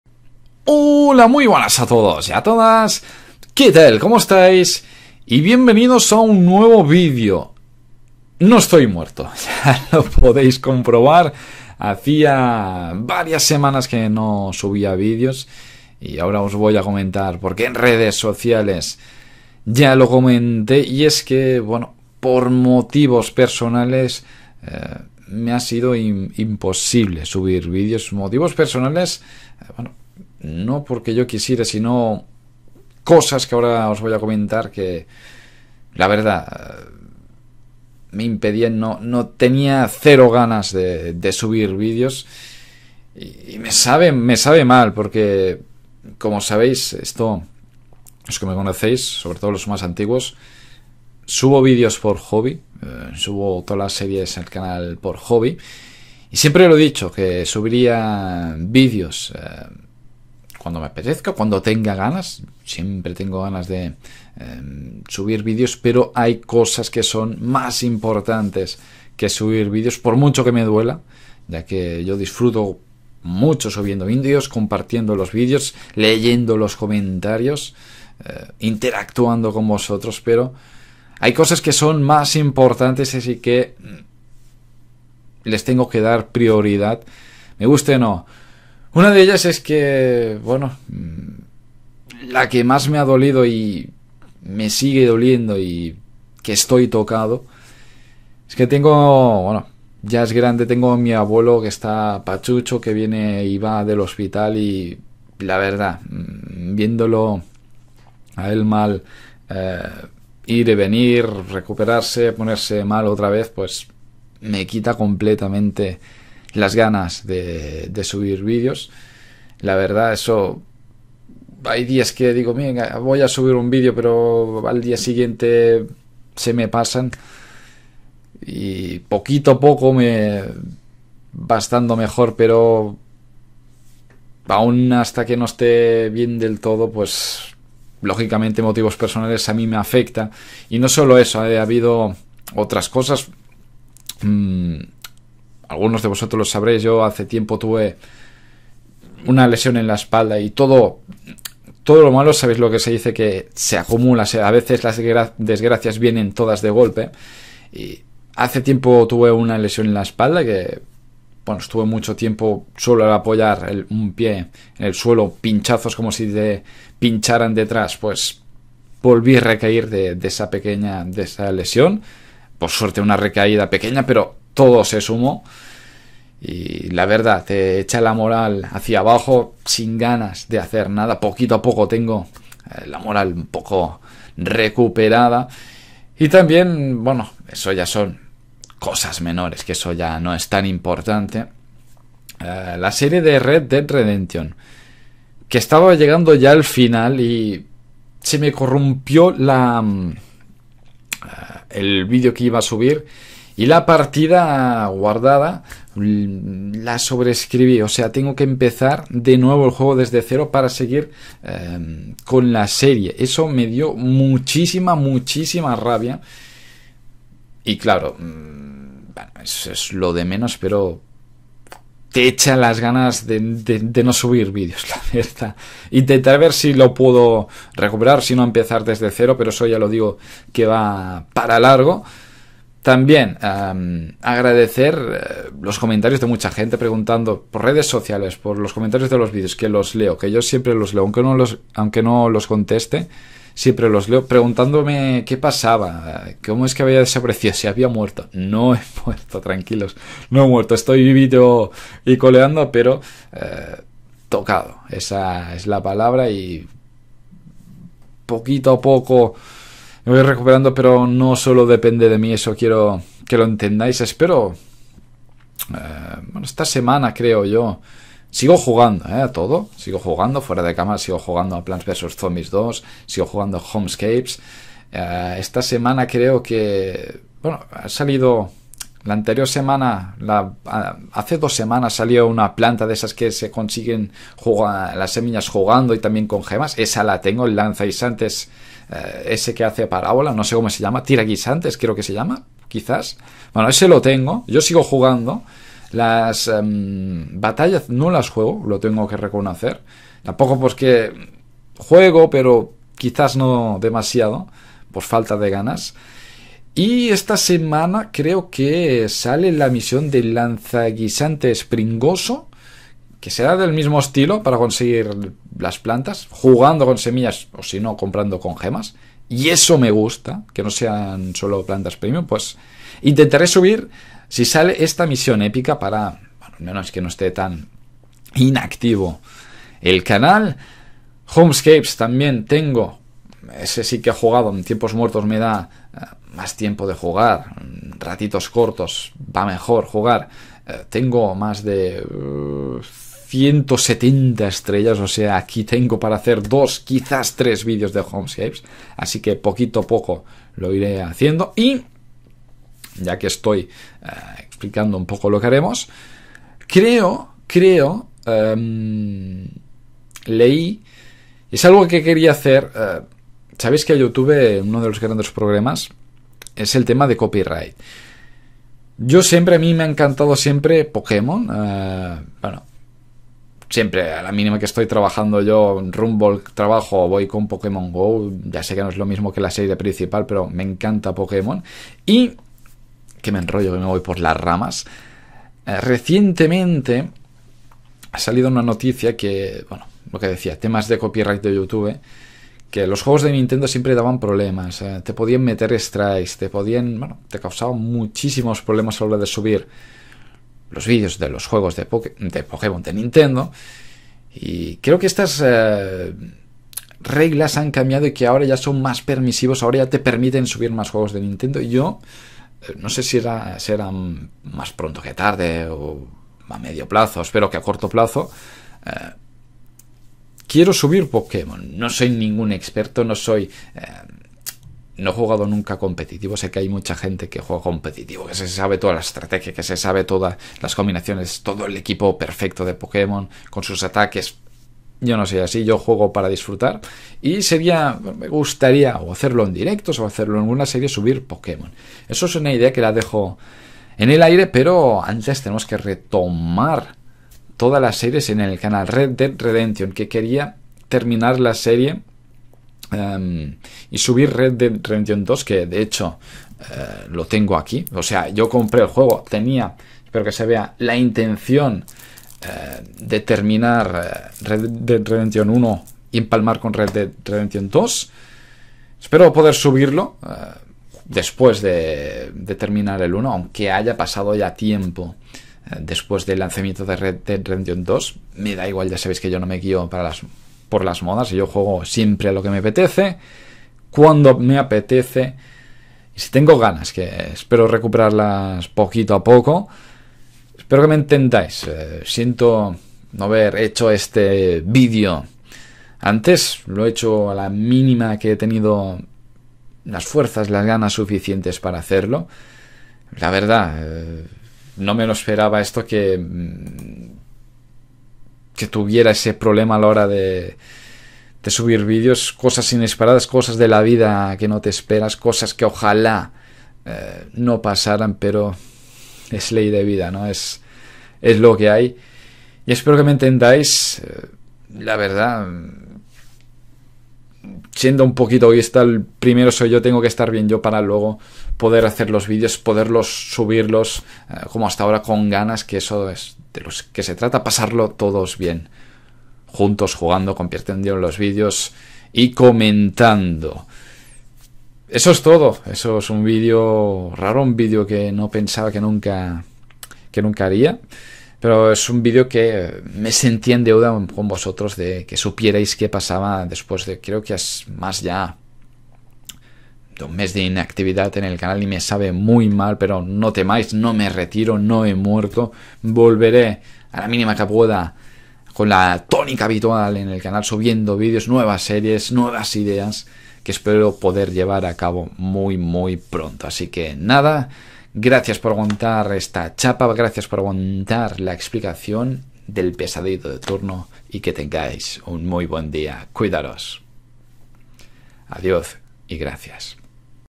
No He Muerto
Video que queria subir hace tiempo comentando el largo tiempo de ausencia en el canal, no temais que no me voy!